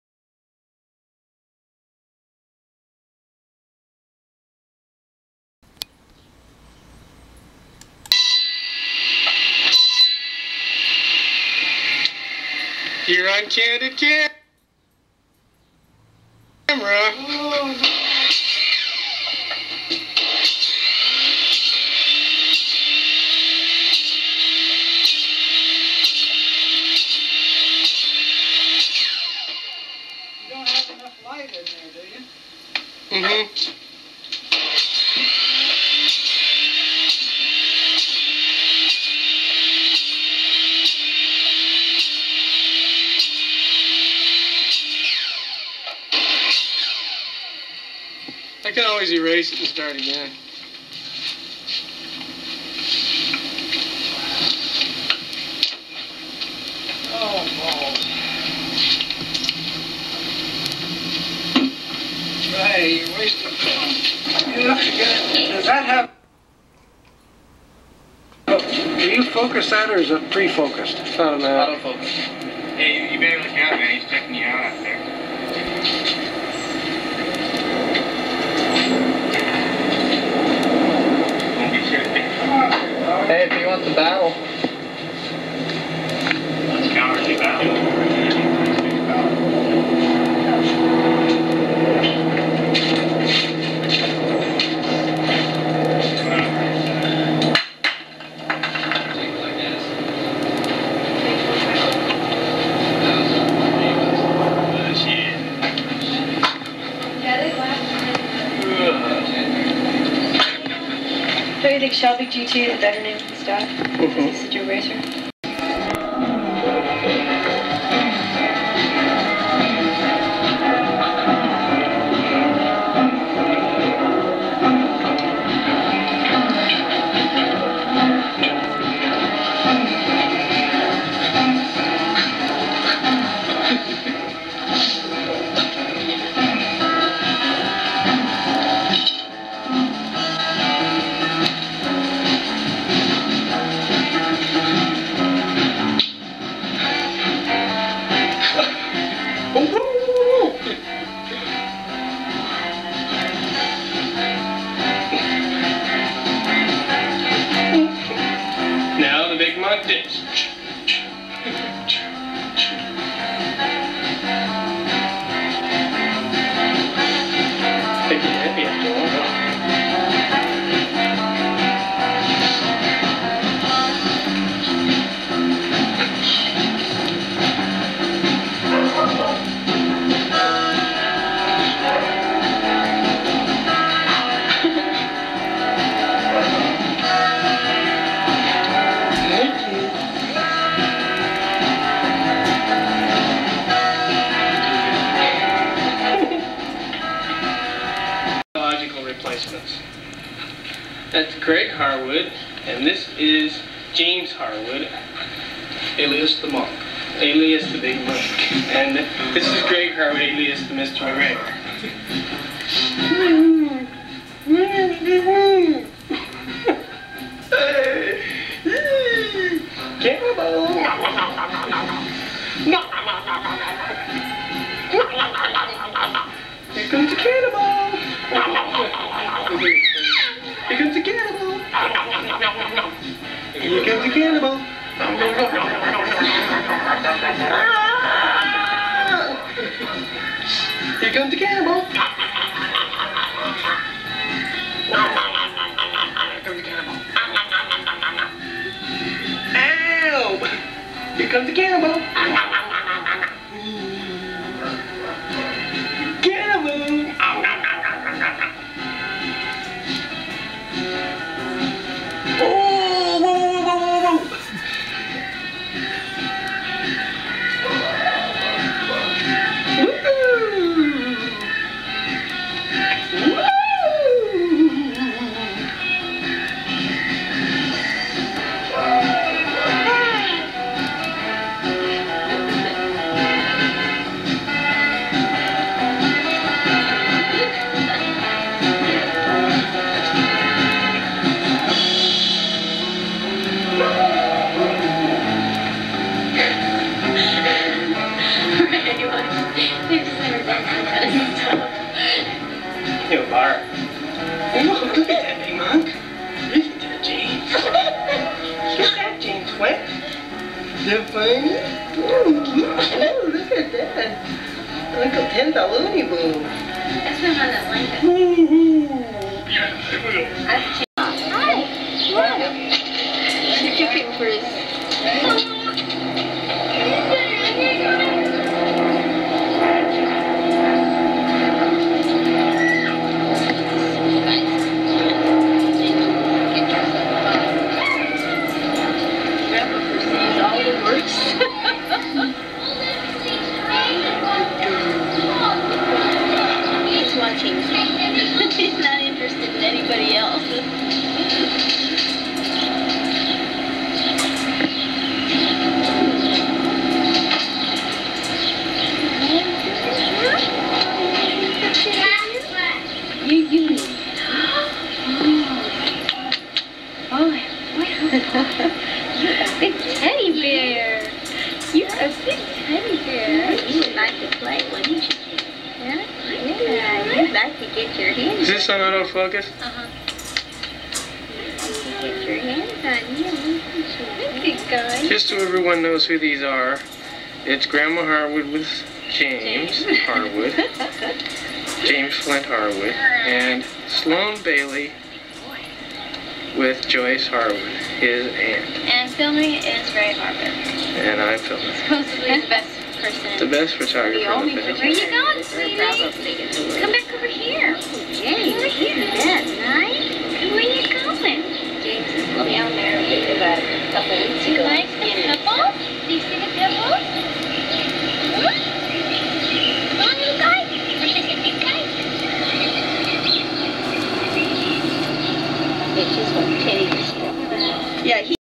HARWOOD FAMILY 1986 # 1.wmv
TED AND NANCY HARWOOD'S HOUSE. SUMMER 1986  Van Nuys, California
 PART 1